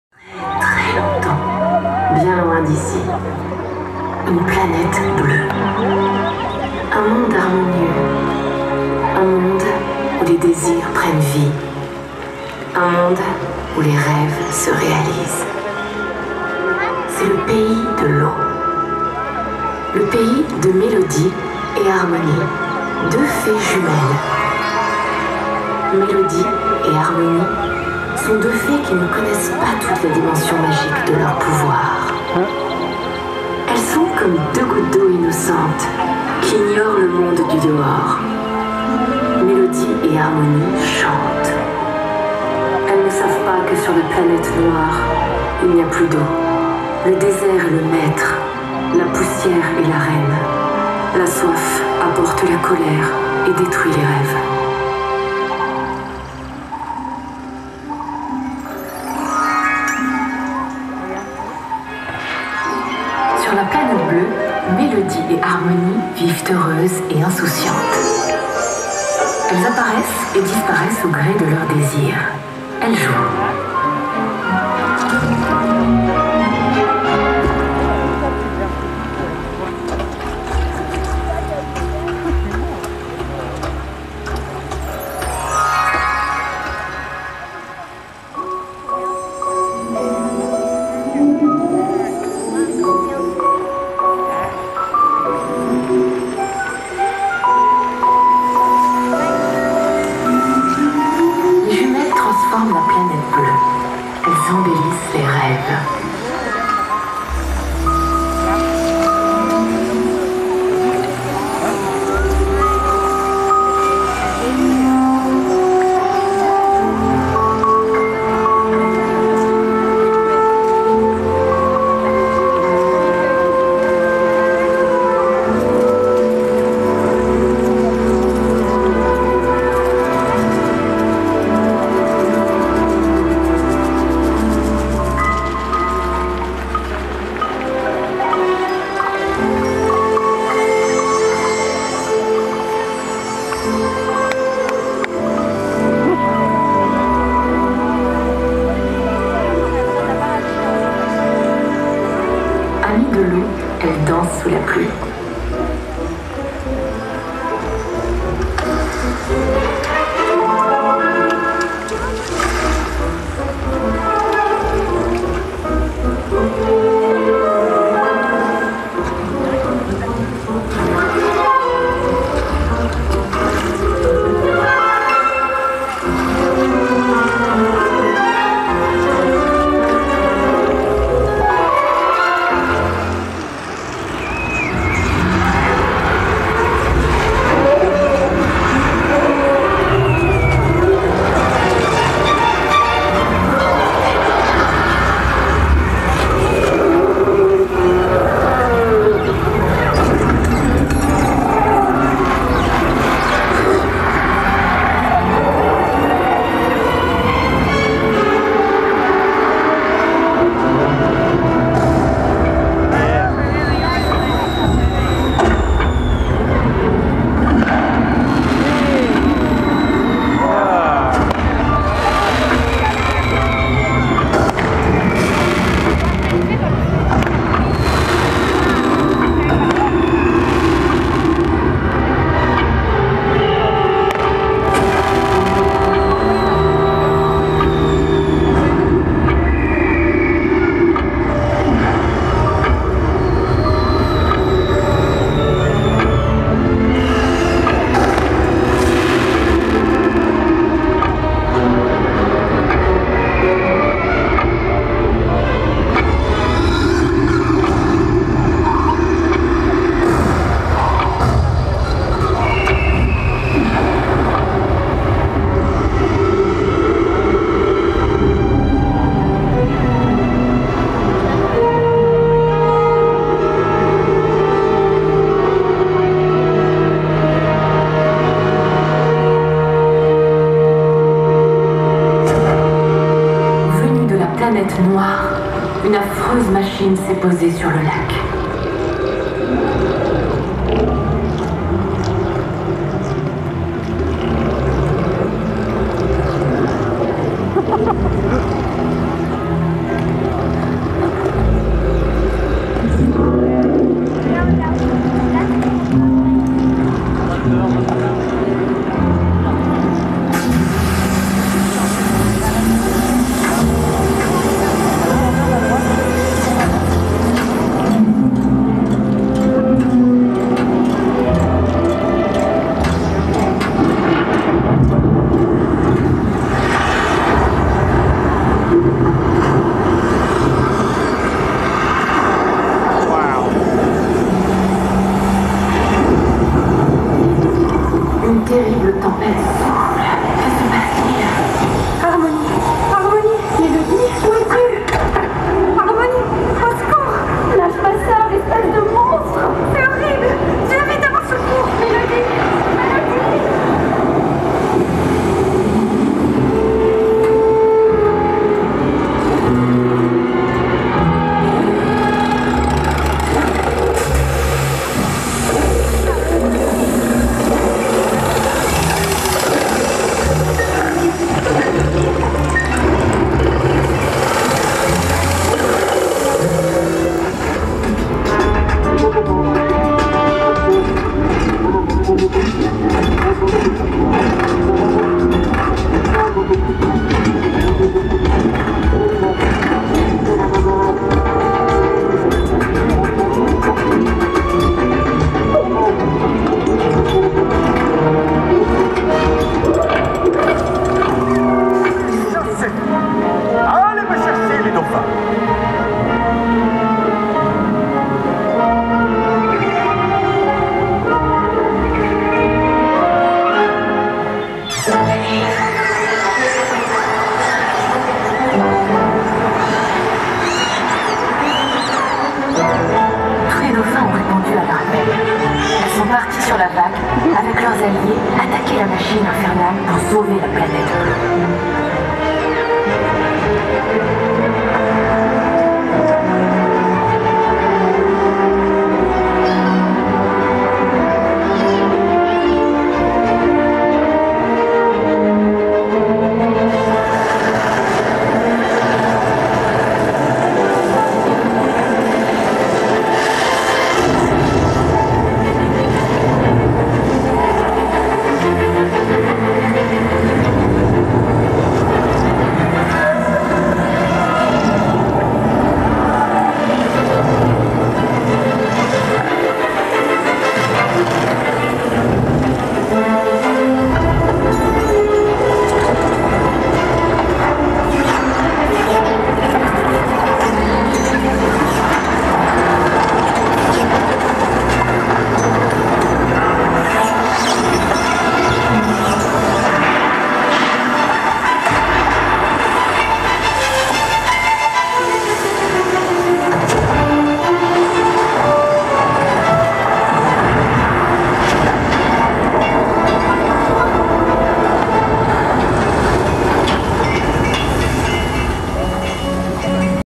Futuroscope's evening 'Lady O' show compilation
4 videos joined as a compilation of the Lady O show at Futuroscope theme park in France. Story of twin sisters from a waterworld that is under threat from an alien invasion using special audio, visual and aquatic effects created by Skertzò.